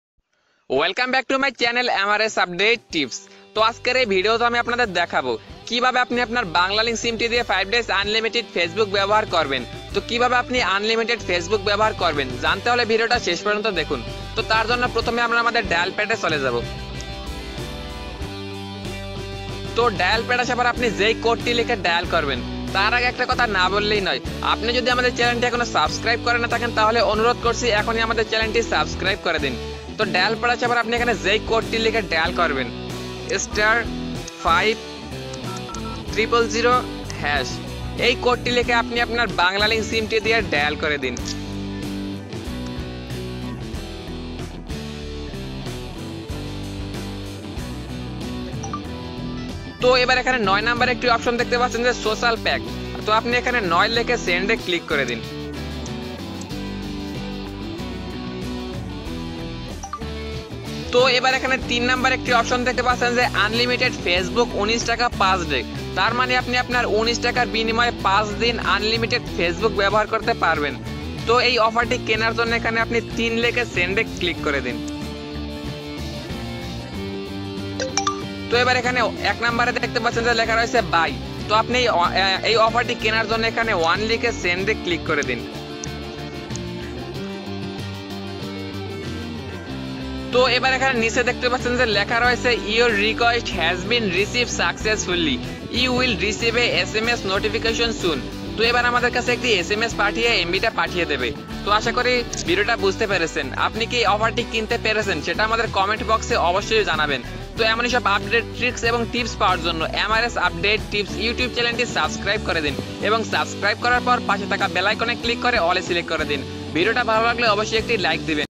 Banglalink Unlimited Facebook pack || Bl free fb | free facebook কিভাবে চালাবো
আপনি আপনার বাংলালিংক সিম দিয়ে Unlimited Facebook ব্যস্ত করুন যার মেয়াদ থাকবে পাঁচ দিন এবং এই এই unlimited facebook ব্যবহার করার জন্য আপনাকে মূল্য দিতে হবে মাত্র ১৯ টাকা যলদি করুন।😱🤗